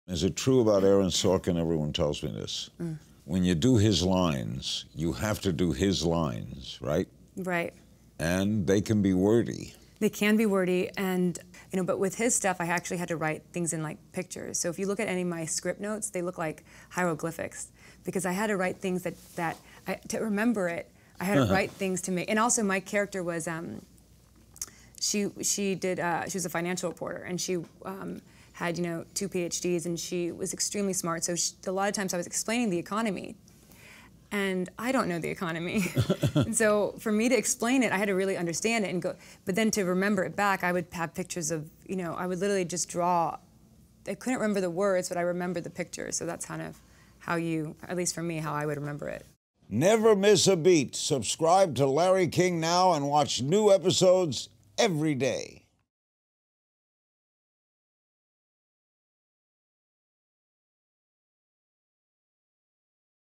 Olivia Munn explains how she memorized her “wordy” scripts for ‘The Newsroom’
While working with Aaron Sorkin on ‘The Newsroom’, Olivia Munn tells Larry exactly how she remembered all her lines from the dense scripts.